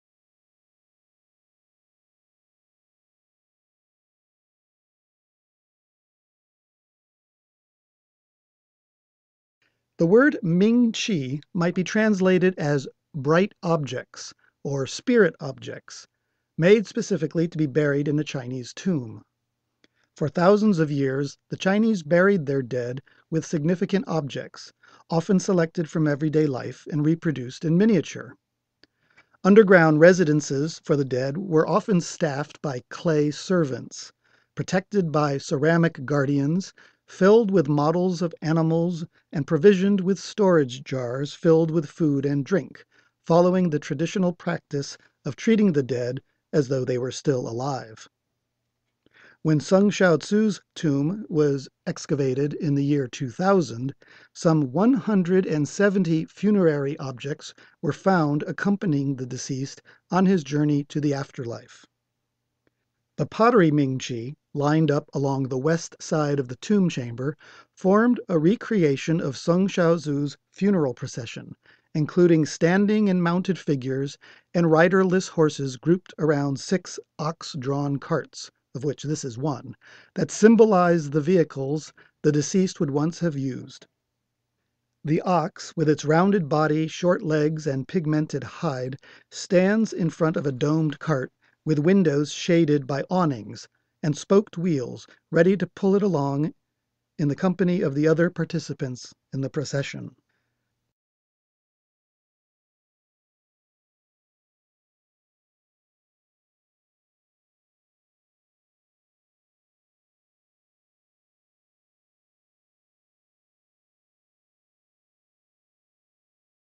Ox Cart, Northern Wei dynasty (386--535 CE)
Unearthed: Recent Archaeological Discoveries from Northern China
Exhibition on view at the Clark June 16, 2012 - October 21, 2012

Ox Cart
Northern Wei dynasty (386--535 CE), tomb dated 477 CE
Painted earthenware, length: 32.6 cm
Unearthed 2000, tomb of Song Shaozu (d. 477 CE), Caofulou Village, Datong, Shanxi Province
Shanxi Museum, Taiyuan